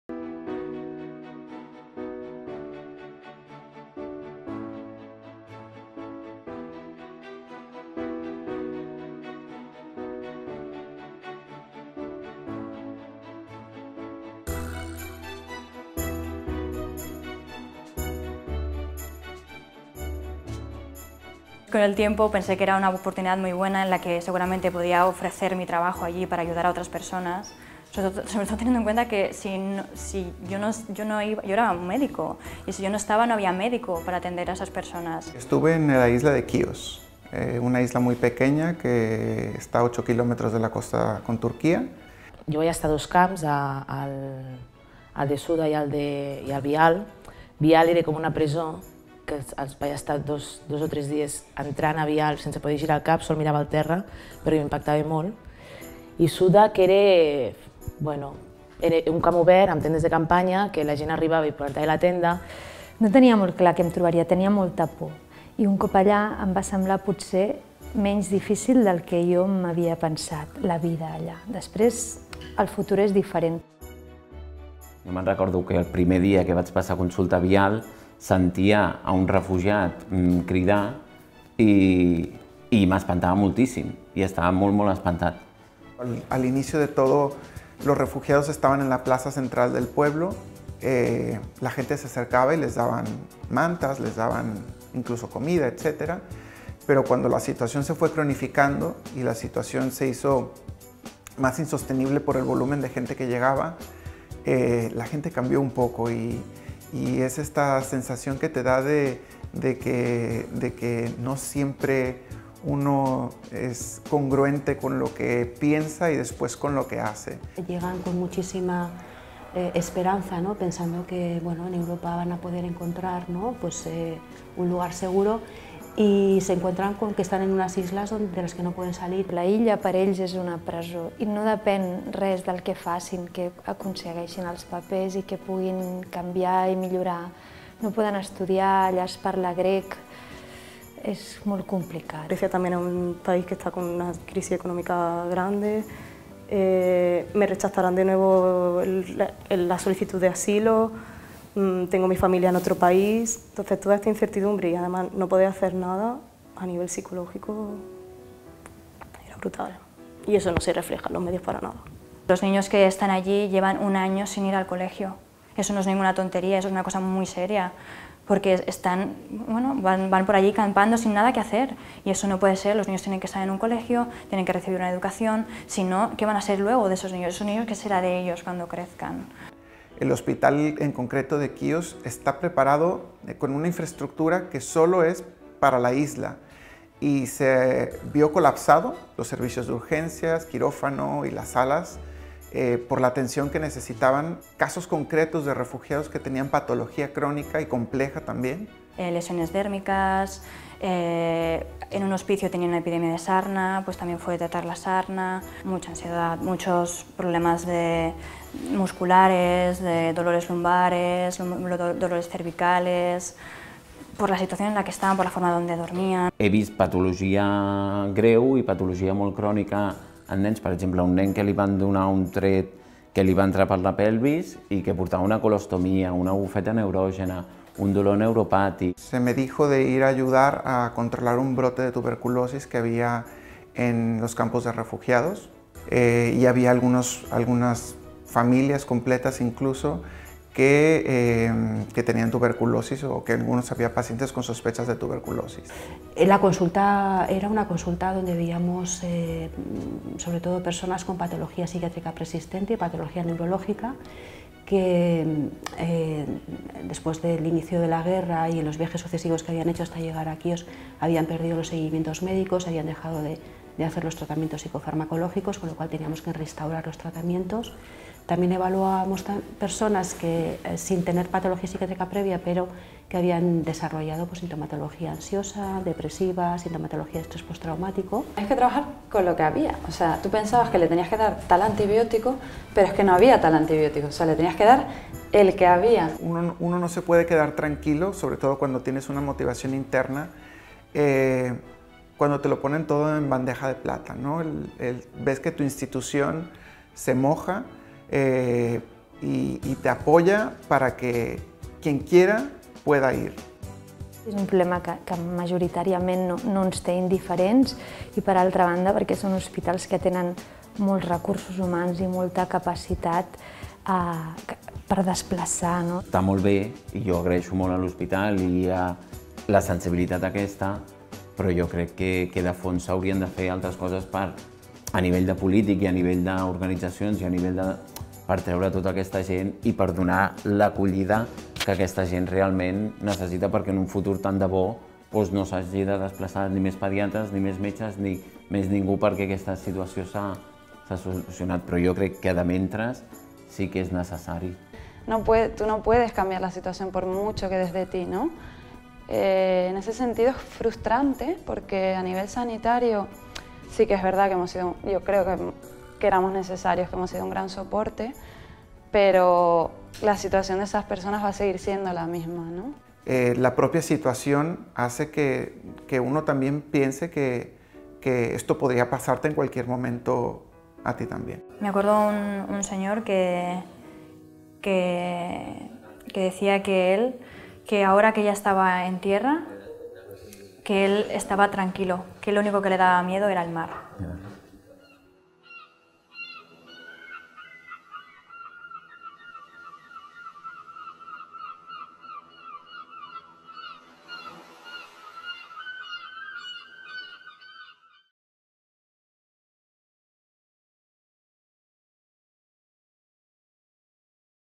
S.O.S. Refugiats. Balanç d'un any d'activitat a Grècia
Vídeo on es recullen les experiències dels cooperants del CAPSBE, Clínic i Sant Joan de Deu que van estar prestant ajuda humanitària a Grècia a través del programa S.O.S. Refugiats